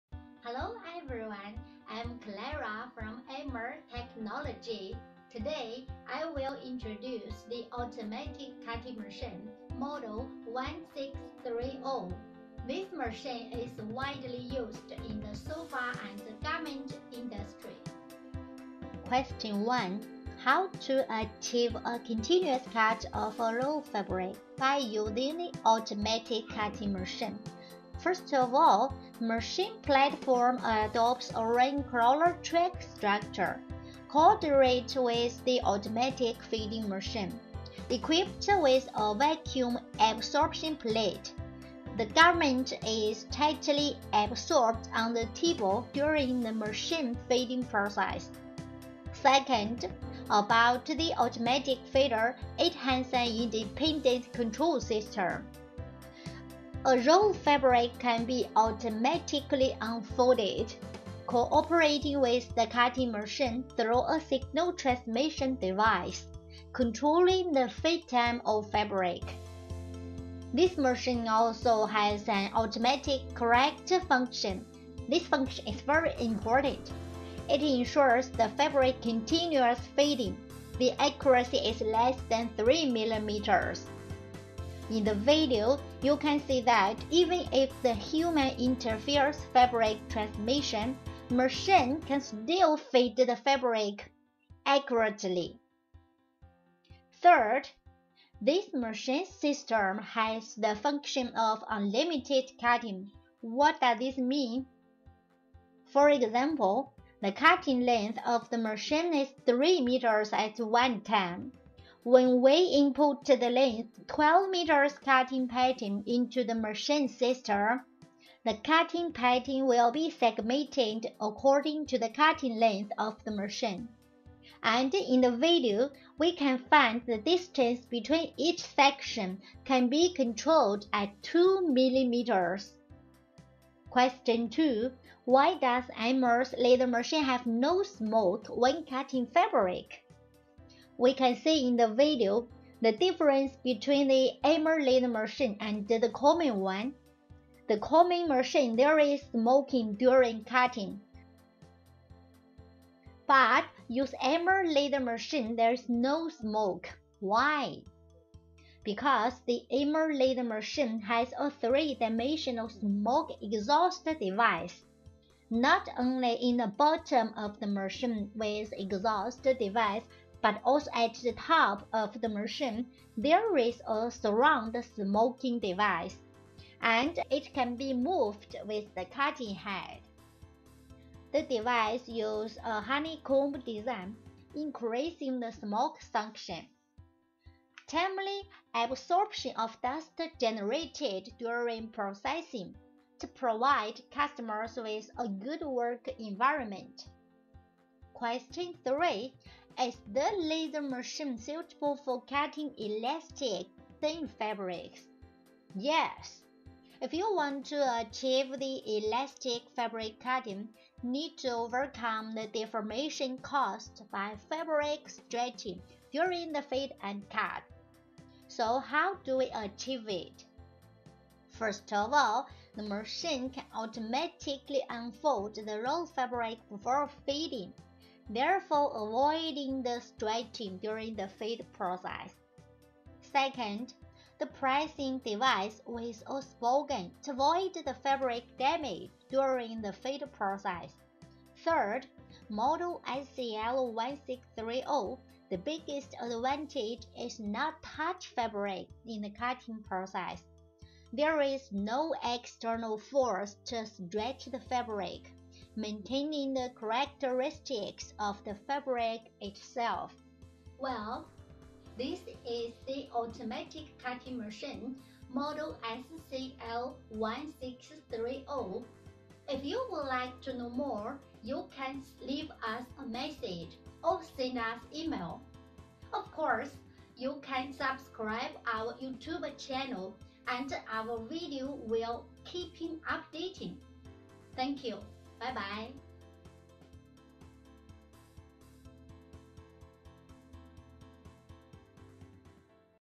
Fabric laser cutting machine|Automatic fabric cutting |Laser cut fabric
Hello, Everyone.I am Clara from AMOR Technology. Today I will introduce the automatic cutting machine model SCL 1630
This machine is widely used in the sofa and garment industry.
Here are three important questions, let us watch the video.
Question 1: How to achieve a continuous cut off a rolled fabric by using the automatic cutting machine?
First of all, the machine platform adopts a ring crawler track structure, coordinate with the automatic feeding machine. Equipped with a vacuum adsorption plate, the garment is tightly adsorbed on the table during the feeding process.

Second, about the automatic feeder, it has an independent control system, a rolled fabric can be automatically unfolded, cooperating with the cutting machine through a signal transmission device, controlling the feed time of fabric.
This machine also has an automatic correction function. This function is very important!!! It ensures the fabric continuous feeding, the accuracy is less than 3mm.

In the video, you can see that even if the human interferes fabric transmission, the machine can still feed the fabric accurately.

Third, this machine’s system has the function of "Unlimited cutting". What does this mean?
For example, the cutting length of the machine is 3 meters at one time.
When we input the length 12meters cutting pattern into the machine system, the cutting pattern will be segmented according to the cutting length of the machine, and in the video, we can find the distance between each section can be controlled at 2mm.

Question2: Why does AMOR's laser machine have no smoke when cutting fabric?
We can see in the video:
The difference between the AMOR laser machine and the common one.

The Common machine, there is smoke during cutting.

But AMOR laser machine cutting, No smoke.
  Why?

Because the AMOR laser machine has a three-dimensional smoke exhaust device. Not only in the bottom of the machine with exhaust device, but also at the top of the machine, there is a surround smoking device. And it can be moved with the cutting head;
The device uses a honeycomb design, increasing the smoke suction.
Timely absorption of dust generated during processing, to provide customers with a good work environment.

Question3: Is the laser machine suitable for cutting elastic, thin fabrics?
Yes.
If you want to achieve the elastic fabric cutting, need to overcome the deformation caused by fabric stretching during the feed and cut.
So how do we achieve it?
First of all, the machine can automatically unfold the roll fabric before feeding, thereby avoiding the stretching during the feed process.

Second, the pressing device with a sponge to avoid the fabric damage during the feed process.

Third, Model SCL-1630, the biggest advantage is not touched fabric in the cutting process .there is no external force to stretch the fabric, maintaining the characteristics of the fabric itself.

Well, this is the SCL1630 automatic cutting machine.

We are a professional sofa automatic fabric cutting machine supplier, with more than 300 sofa factories using our equipment.